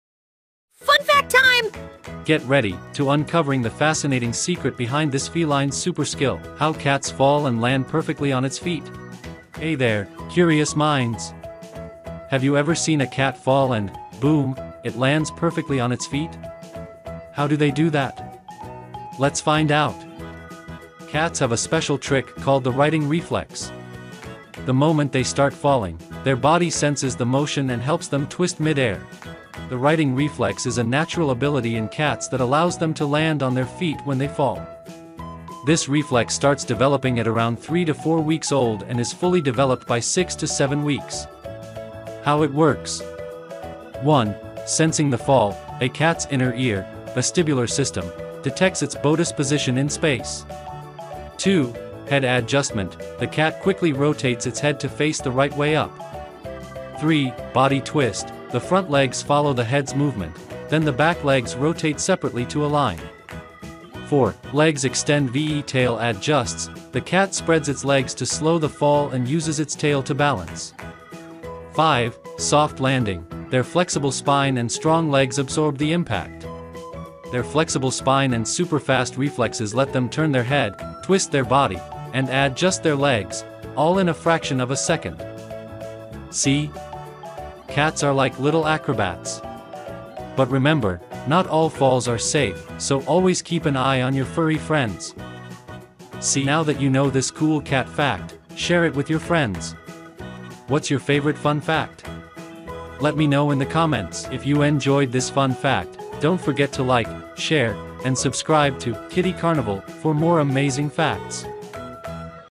FunFact : Why Cats Always Land Like Ninjas | Amazing Cat Reflex!
Hey, everyone welcome 🙏  to " kiddie carnival ".

Have you ever seen a cat fall and somehow land perfectly on its feet? It’s not magic—it's science!
In this fun and fascinating video, discover how cats use the incredible righting reflex to twist mid-air and land safely. Watch step-by-step visuals and learn what makes cats such agile and graceful creatures.